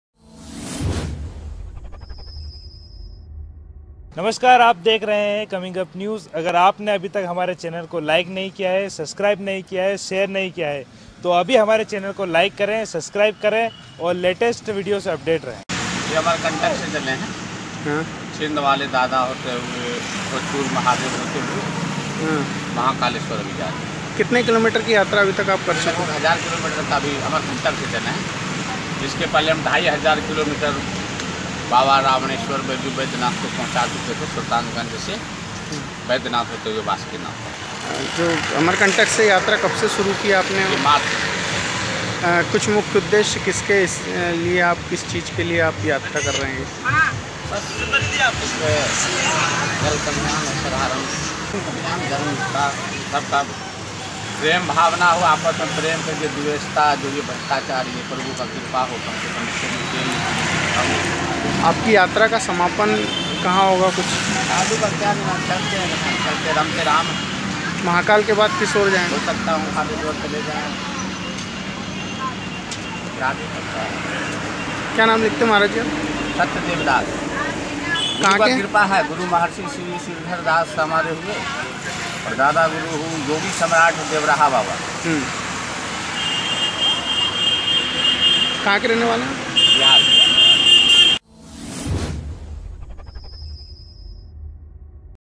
अमरकंटक के संत का Baba Mahakal से लगाव | भक्ति में कर दिया ये अनोखा काम | TYAGI BABA YATRA
ComingUp News---  बुजुर्ग...लेकिन संतुलित काया, सिर पर लाल पगड़ी व शरीर पर महज एक लंगोट और हाथ में लकड़ी का छोटा टुकड़ा लिए एक बाबा जब शहर की सड़कों पर दंडवत (जमीन पर पेट के बल लेटना) होकर आगे बढ़ते हुए दिखे, तो लोगों के लिए नजारा असाधारण था। पूछने पर पता चला कि ये सत्यदेवदास महाराज हैं, जो कुछ किलोमीटर नहीं, बल्कि अमरकंटक से इसी तरह दंडवत चलते हुए महाकाल दर्शन  के लिए उज्जैन पहुंचे हैं। उनके अनुसार 15 महीने की यात्रा में वे करीब 3 हजार 500 किलोमीटर चल चुके हैं।